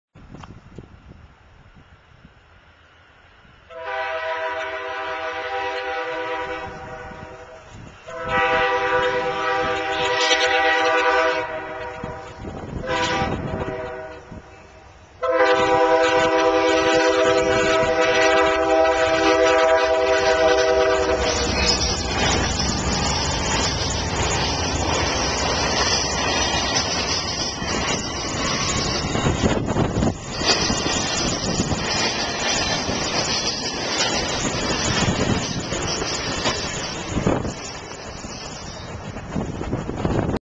Amtrak Southwest Chief #3 in Canyoncito
Amtrak Southwest Chief #3 heads west through Canyoncito on the way to its next station stop of Lamy, NM.  Recorded on February 27, 2011.